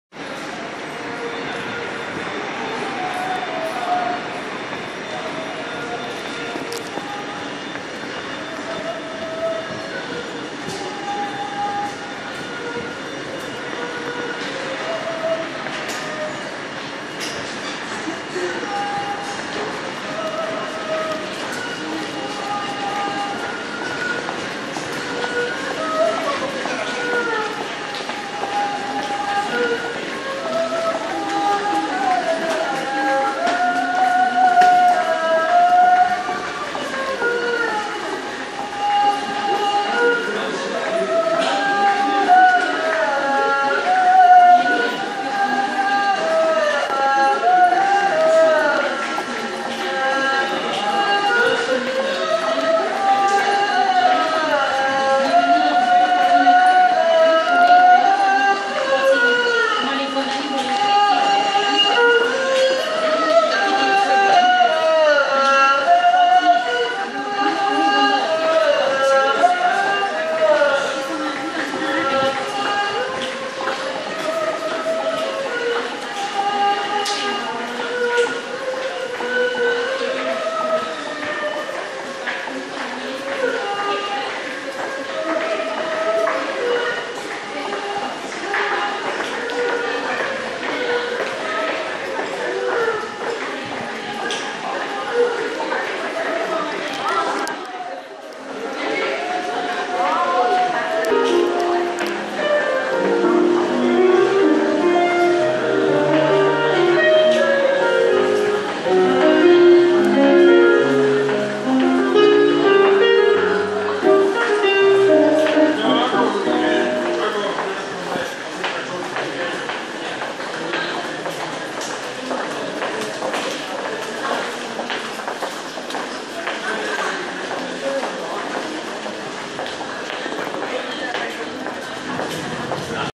The subway connection at Central Station Sydney | vlog | Adventure Experience
This subway at Central Station connects from Elizabeth St to George St.